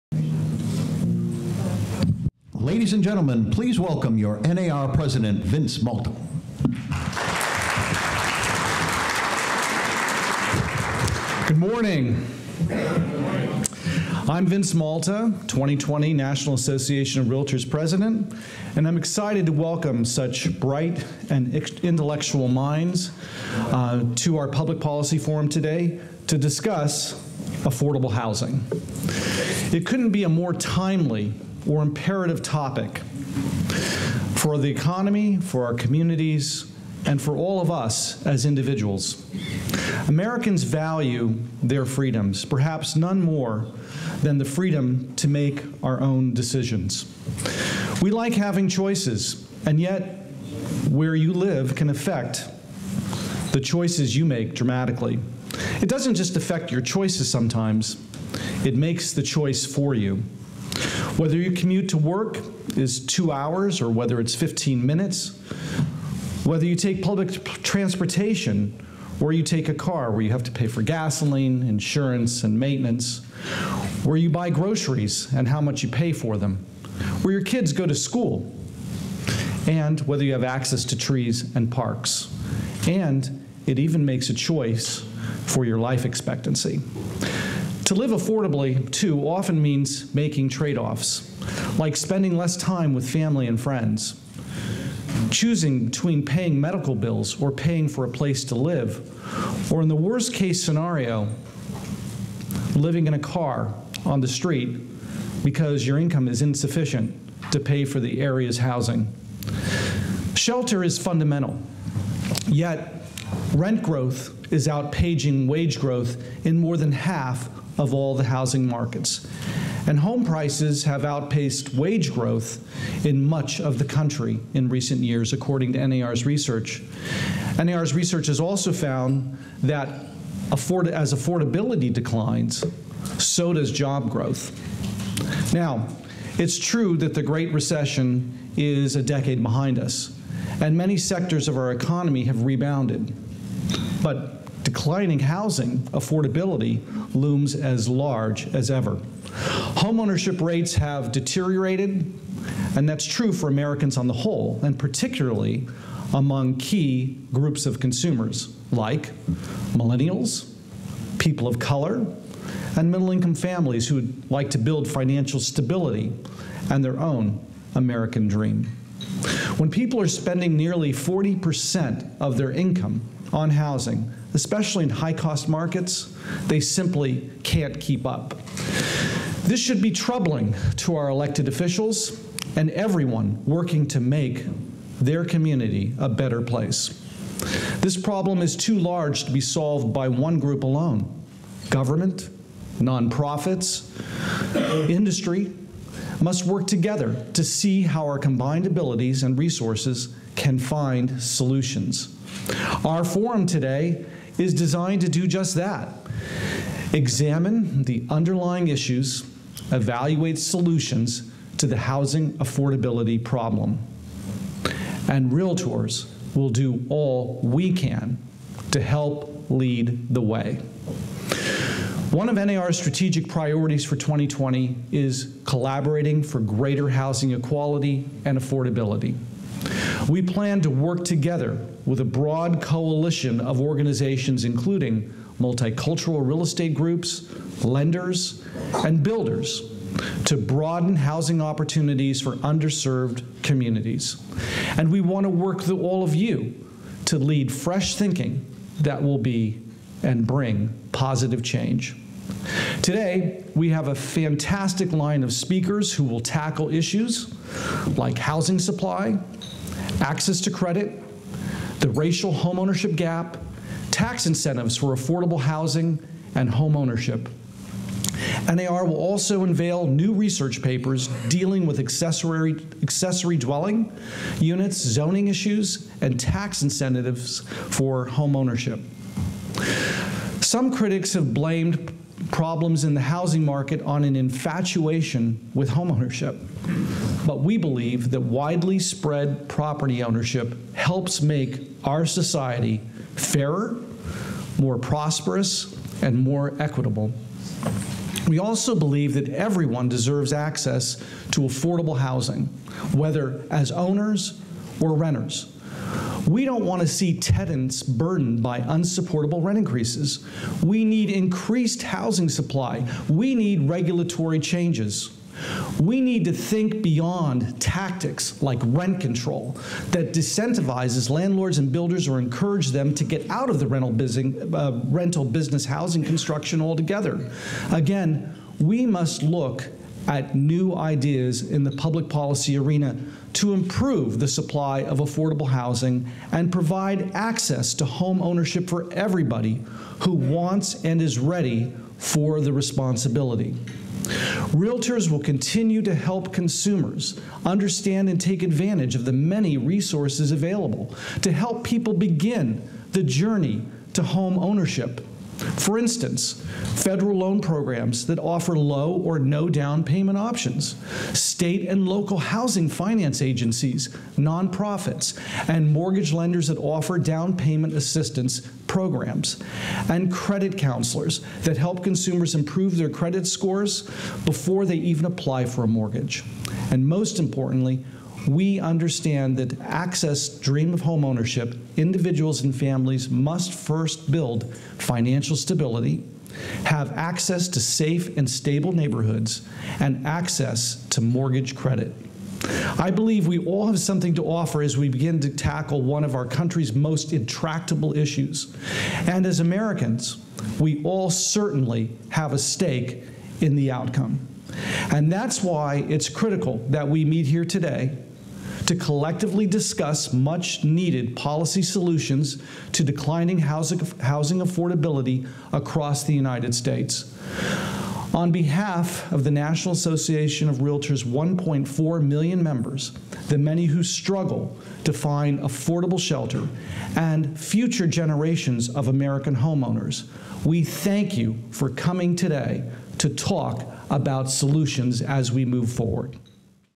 NAR Policy Forum: Welcome Message From 2020 NAR President
Welcome session hosted by Vince Malta, 2020 NAR President.